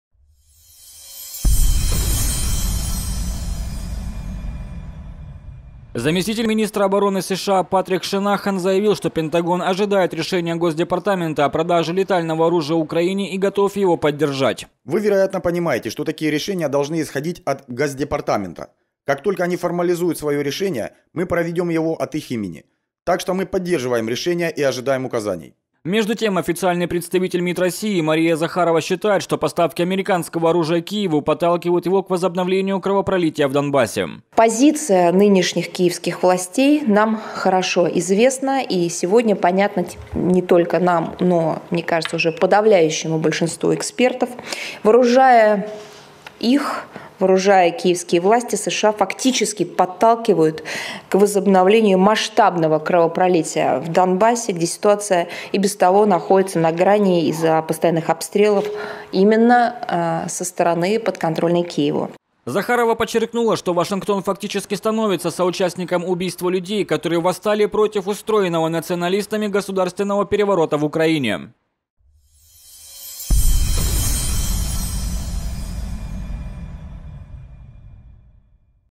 Пентагон ожидает решения о продаже летального оружия Украине
Заместитель министра обороны США Патрик Шенахан заявил, что Пентагон ожидает решения Госдепартамента о продаже летального оружия Украине и готов его поддержать.
"Вы, вероятно, понимаете, что такие решения должны исходить от госдепартамента. Как только они формализуют своё решение, мы проведём его от их имени. Так что, мы поддерживаем решение и ожидаем указаний"
Между тем, официальный представитель МИД России Мария Захарова считает, что поставки американского оружия Киеву подталкивают его к возобновлению кровопролития в Донбассе.
"Позиция нынешних киевских властей нам хорошо известна и сегодня понятна не только нам, но, мне кажется, уже подавляющему большинству экспертов. Вооружая их, киевские власти, США фактически подталкивают к возобновлению масштабного кровопролития в Донбассе, где ситуация и без того находится на грани из-за постоянных обстрелов, именно со стороны, подконтрольной Киеву"
Захарова подчеркнула, что Вашингтон фактически становится соучастником убийства людей, которые восстали против устроенного националистами государственного переворота в Украине.